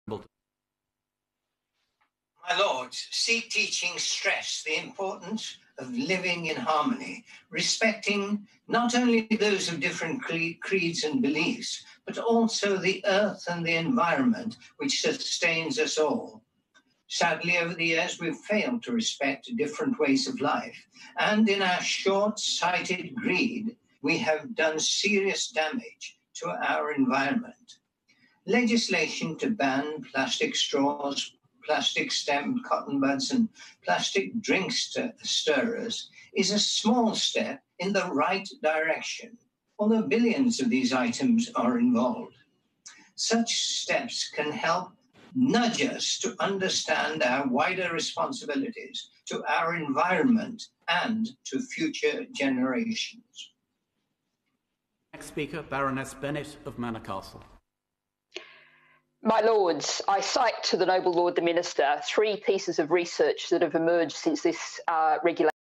Environmental Protection (Plastic Straws, Cotton Buds and Stirrers) Regulations l 10 July 2020
Lord Singh of Wimbledon's contribution on the Environmental Protection (Plastic Straws, Cotton Buds and Stirrers) (England) Regulations 2020 debate in The House of Lords